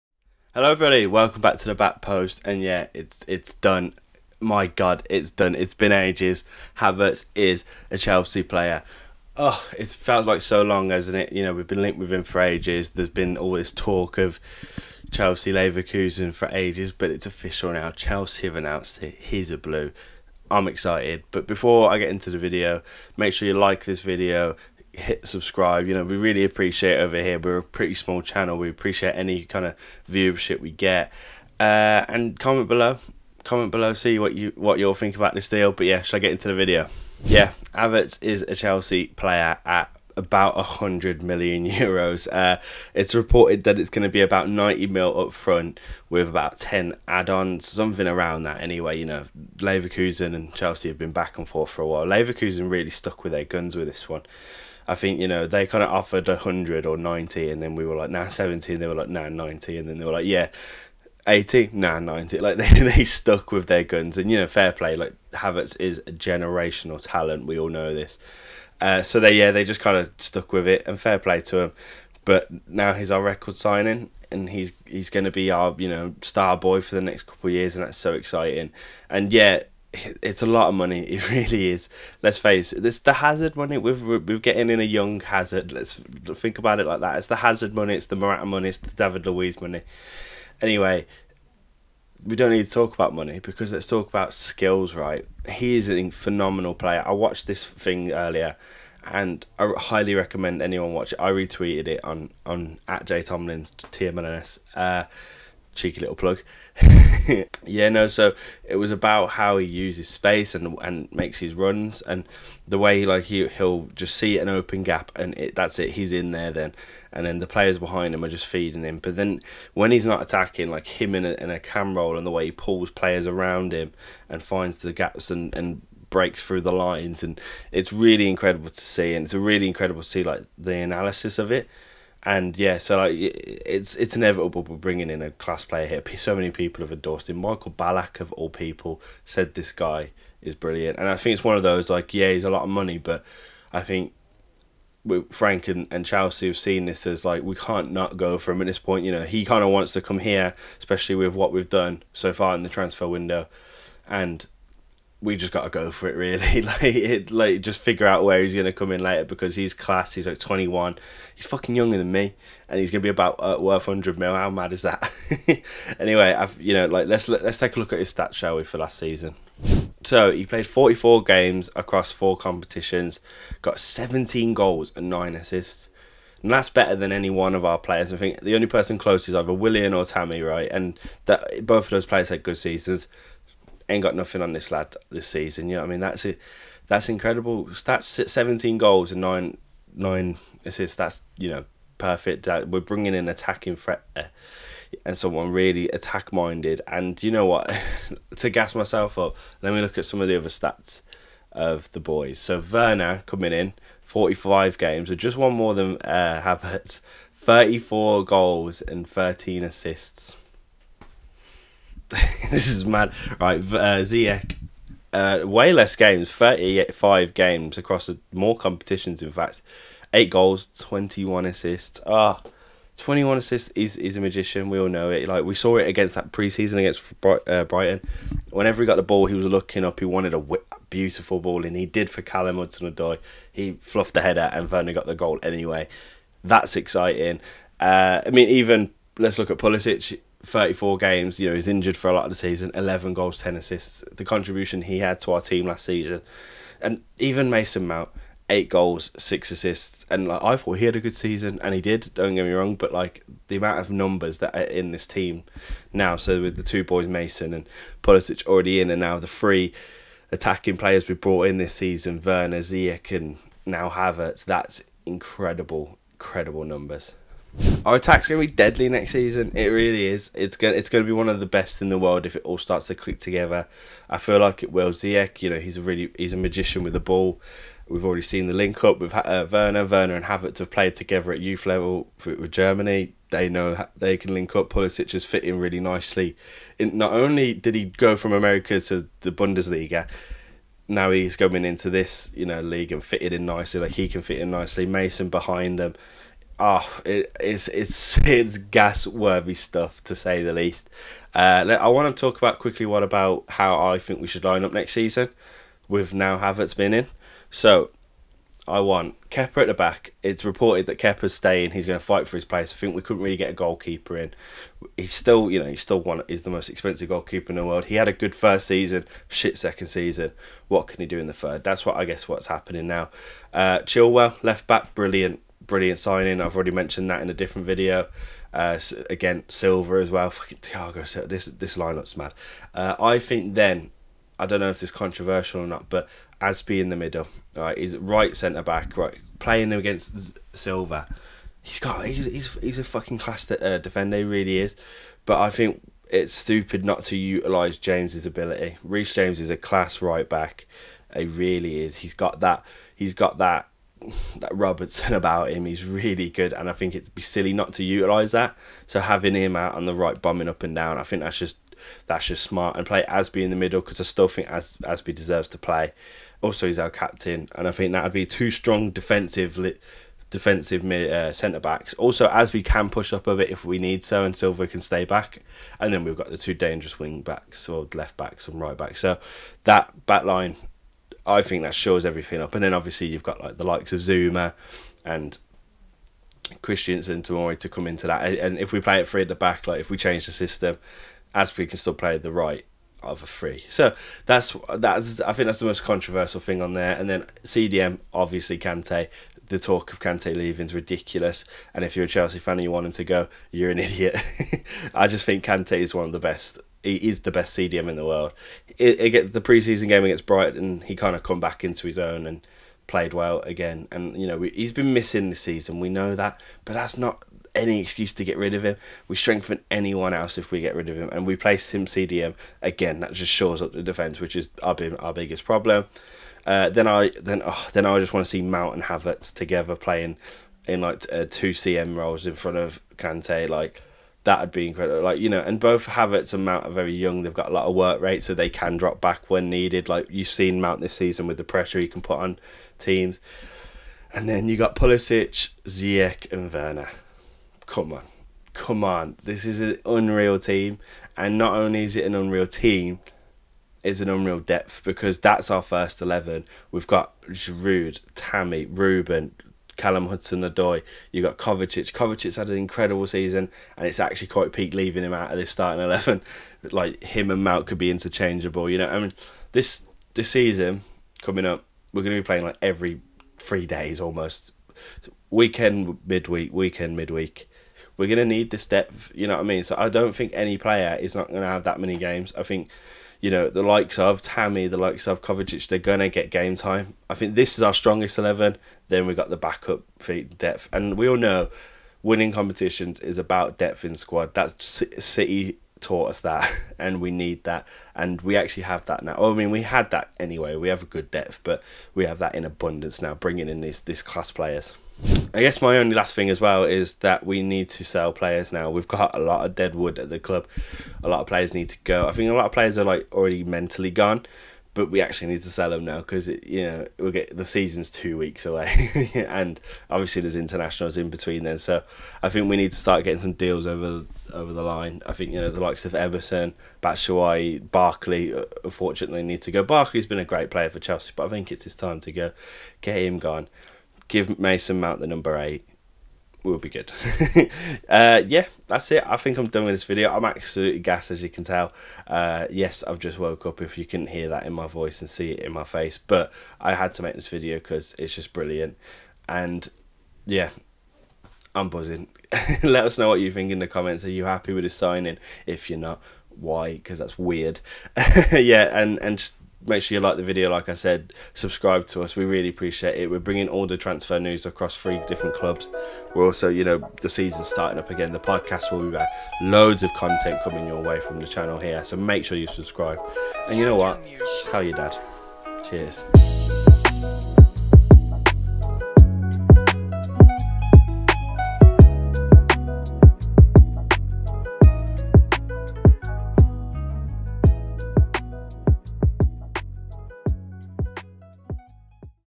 WELCOME TO CHELSEA KAI HAVERTZ! // CHELSEA TRANSFER NEWS [ REACTION ]
Chelsea have signed Kai  Havertz, finally! Chelsea have agreed with Bayer Leverkusen to sign the German youngster! Here's Jays reaction to that!

*I mention in this video the old price. Marina worked her magic and got him for £62 😭💙